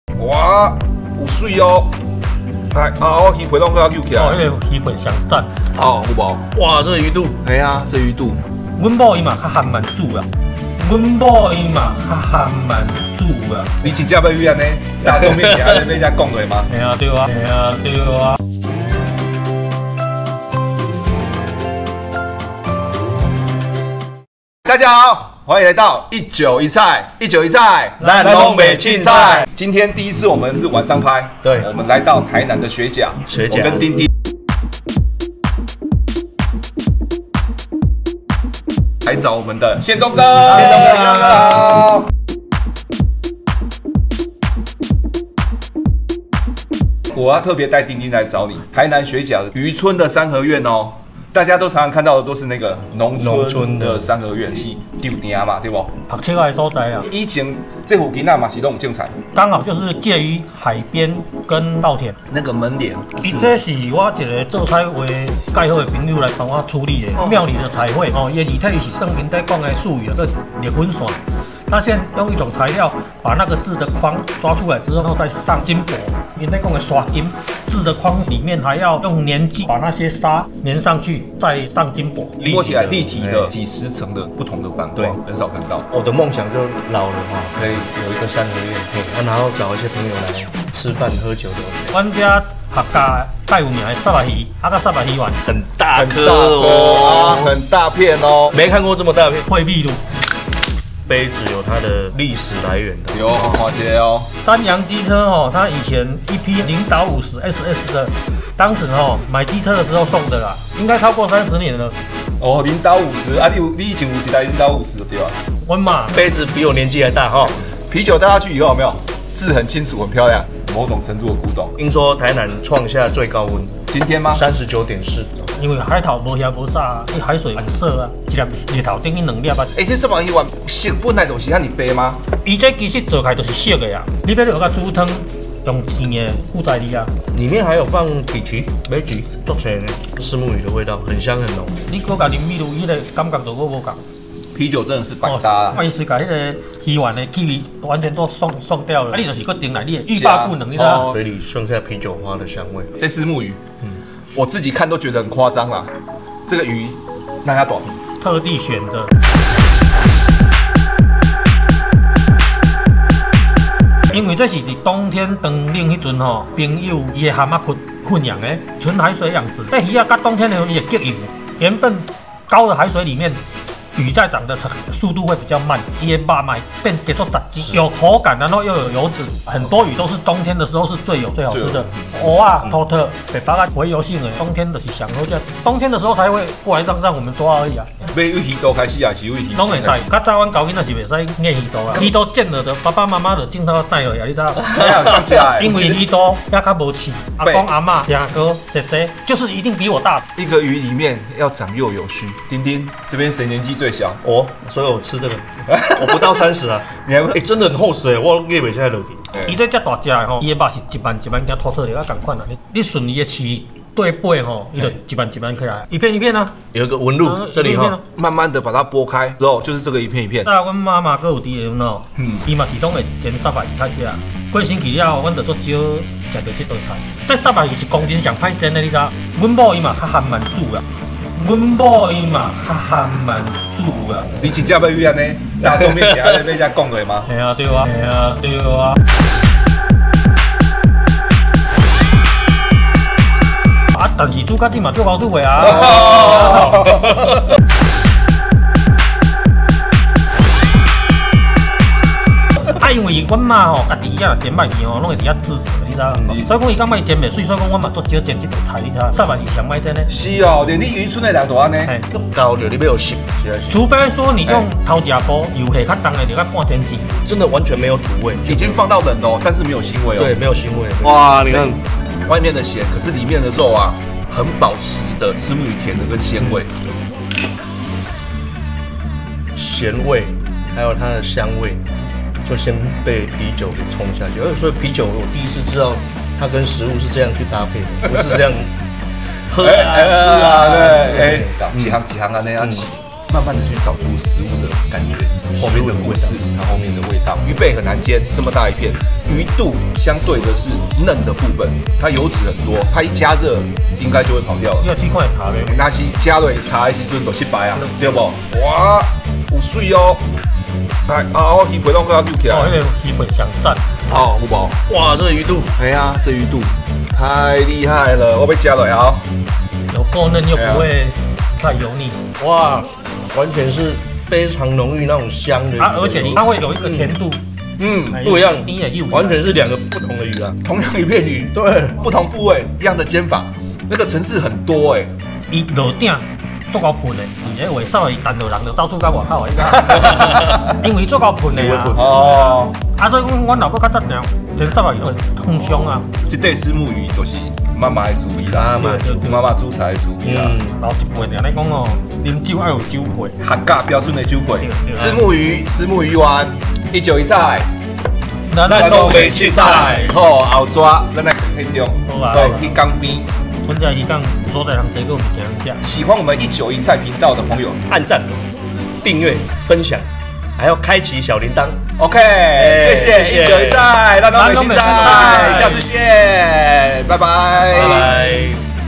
比臉還大片的煎虱目魚肚 竟然隱藏在三合院【一酒一菜1wine1dish】ㄧ酒一菜X啤酒虱目｜1wine1dish times Beer & Milkfish
比臉還大片的煎虱目魚肚，竟然隱藏在三合院？養過冬天的虱目魚，肉質竟然就像土魠魚，一片一片就像魚排？隱藏在台南漁村裡的私密虱目魚料理，一酒一菜大揭密！
一酒一菜邀請好朋友丁丁一起前往拜訪，台南學甲的憲忠哥，憲忠哥親自料理號稱漁村裡最難做的料理，乾煎虱目魚，沒想到魚的SIZE是XXXL，嚇到大家，但滋味卻是超級美味，再配上新鮮的虱目魚丸，暢飲海尼根！就是一個順暢啦！
一酒一菜 咱攏袂襯採